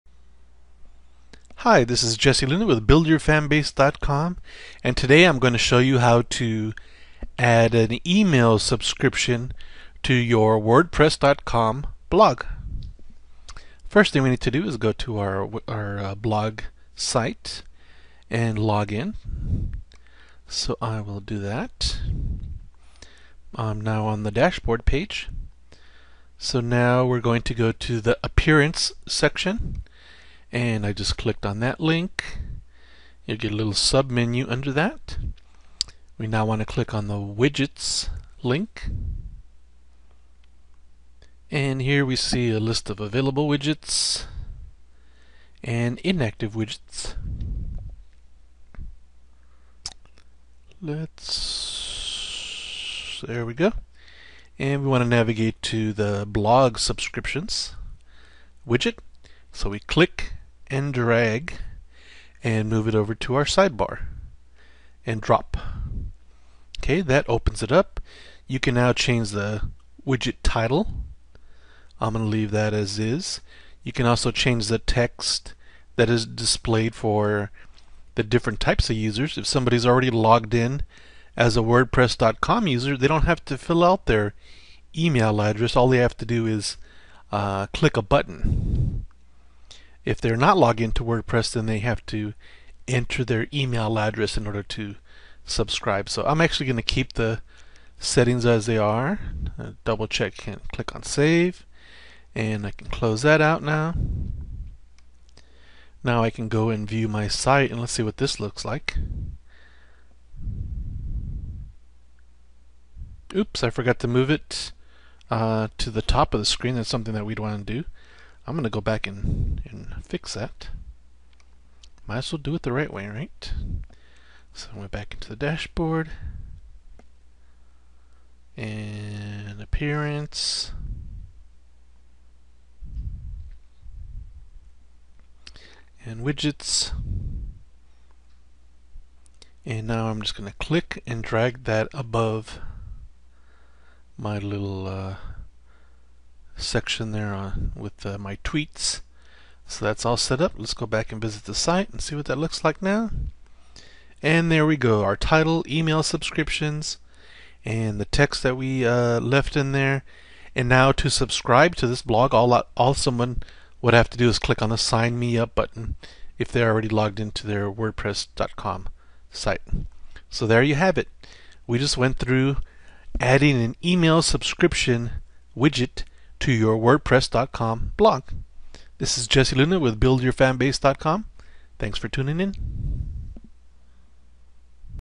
HOW TO Add an Email Subscription Widget to Your WordPress.com Site
HOW TO Add an Email Subscription Widget to Your WordPress.com Site.  WordPress has a nifty new feature that allows its users to add an EMAIL SUBSCRIPTION feature to their blogs!  This is way easier than having to create an account with a third-party company like Feedburner to manage email subscriptions.